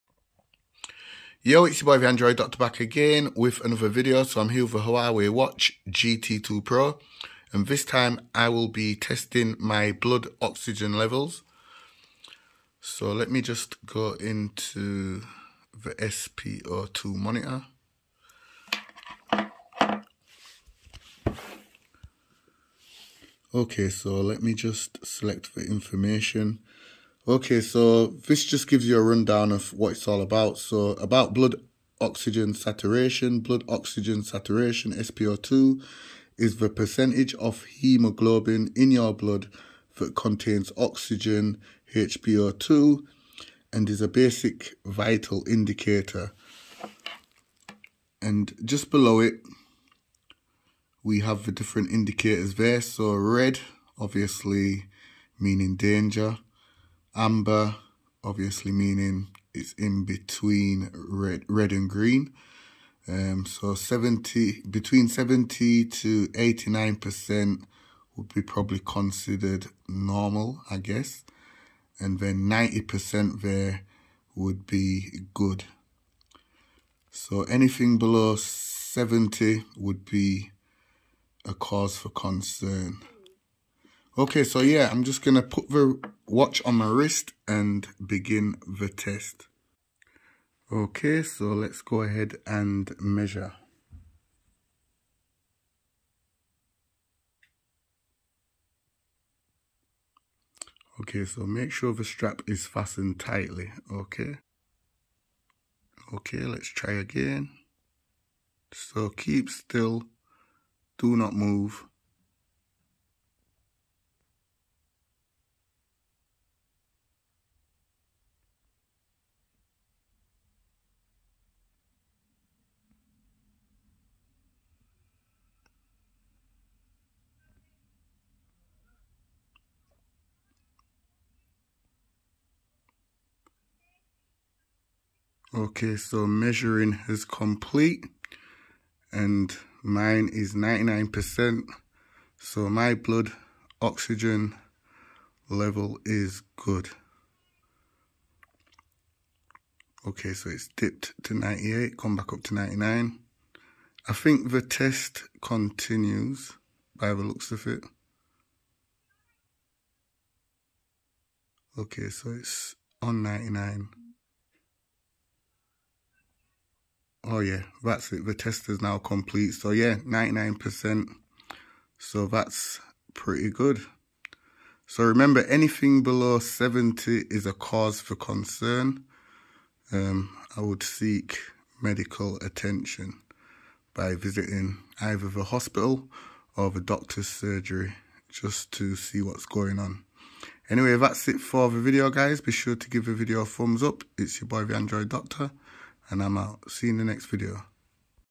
Huawei Watch GT 2 Pro (Sp02) Blood Oxygen Test Measure the % of Haemoglobin in your Blood Oxygen
hey everyone android doctor here again with another video and today i will be using my new huawei watch gt 2 pro to measure my blood oxygen saturation sp02 this is the percentage of haemoglobin in your blood that contains oxygen (Hb02)